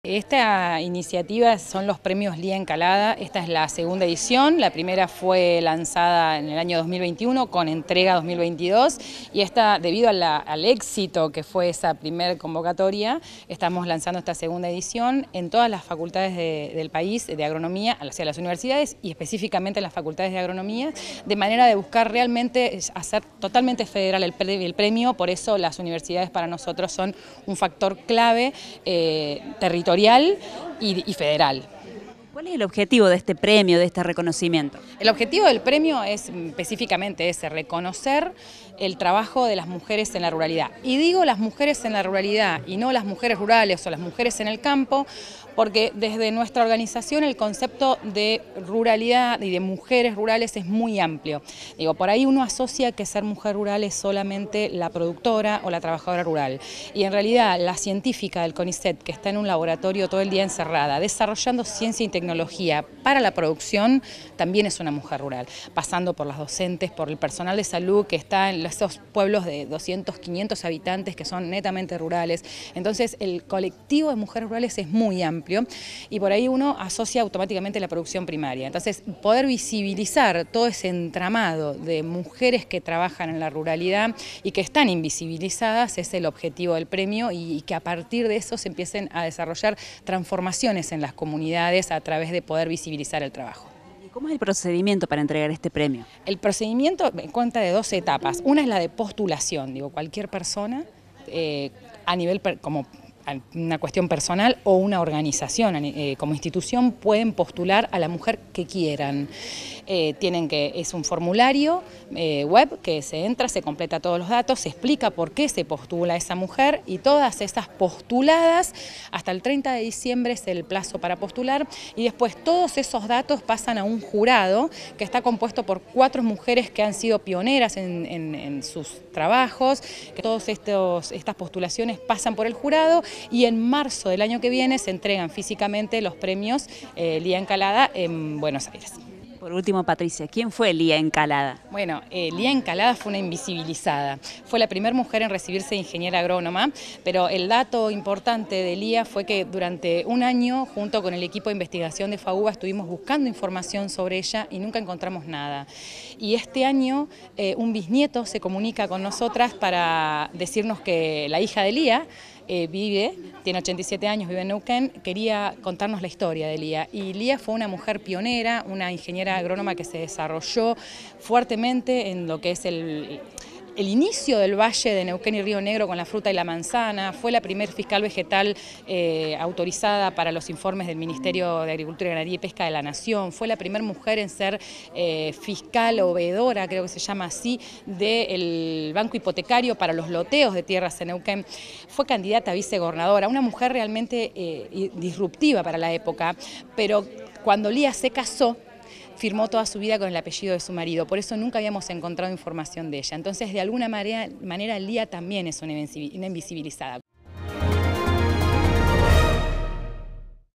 PREMIOS “LÍA ENCALADA 2023”: BUSCAN RECONOCER Y VISIBILIZAR A MUJERES RURALES
PATRICIA GORSA – PRESIDENTA MRA